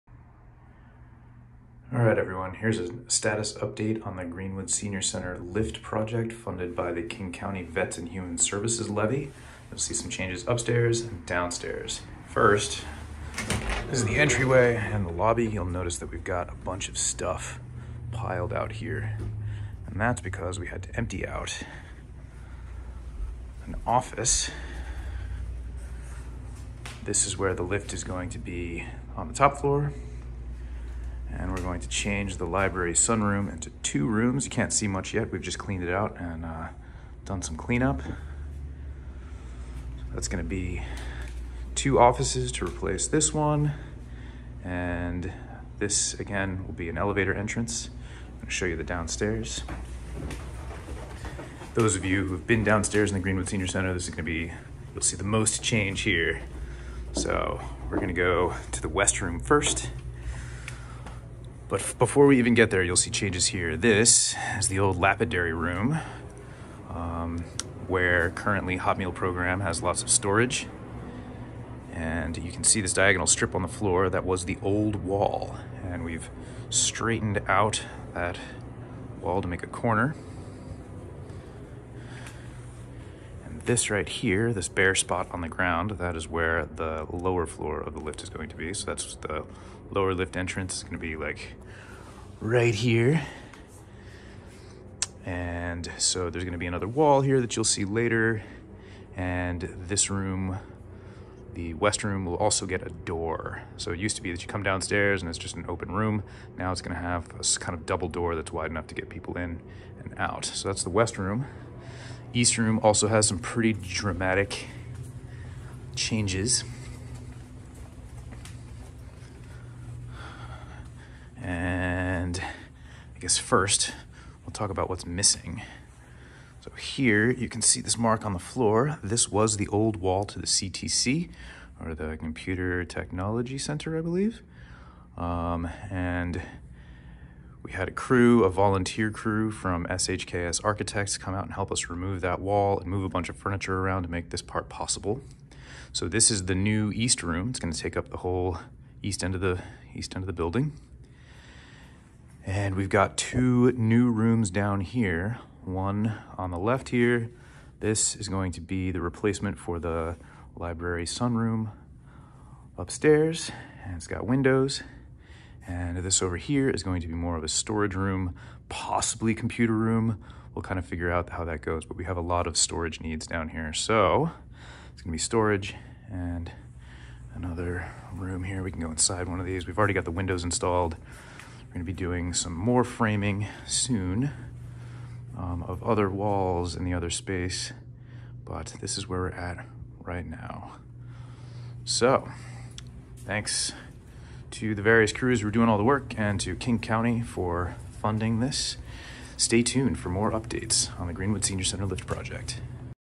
Greenwood Senior Center Lift Project Update 1
In the fall/winter of 2020, PNA partnered with SHKS Architects and G3 builders to install a vertical platform lift and make related space improvements at the Greenwood Senior Center. The project was funded by King County’s Veterans, Seniors, and Human Services Levy.

The work included demolishing an office to make room for the lift itself, installing the lift, reconfiguring office space upstairs, and redesigning the downstairs to make better use of the limited space we have.